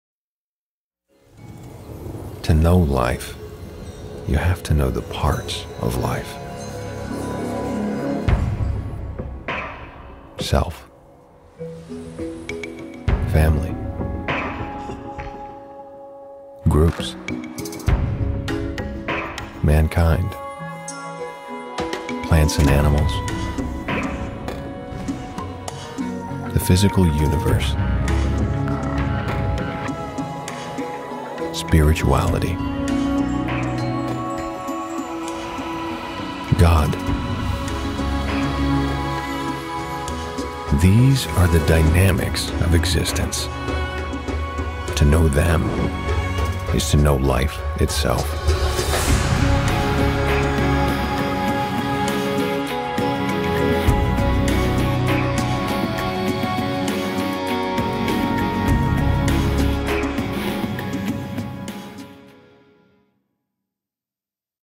Scientology TV Commercial Ad Message: The Dynamics of Existence
A new 60-second TV Commercial Message from the Church of Scientology, called "The Dynamics of Existence."

"To know life, you have to know the parts of life:
Self,
Family,
Groups,
Mankind,
Plants & Animals,
The Physical Universe,
Spirituality,
God.
These are the Dynamics of Existence.
To know them is to know life itself."

Scientology
KNOW YOURSELF, KNOW LIFE

- Scientology TV Commercial Message -- The Dynamics of Existence